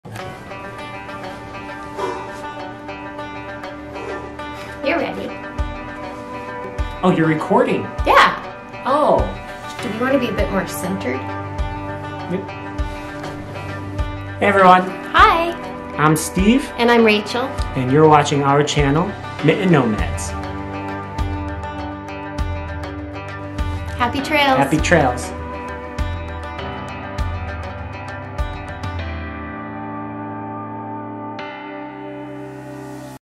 Mitten Nomads - Hi!
Hi all!
We'll be posting DIYs, vlogs and general content around our adventures.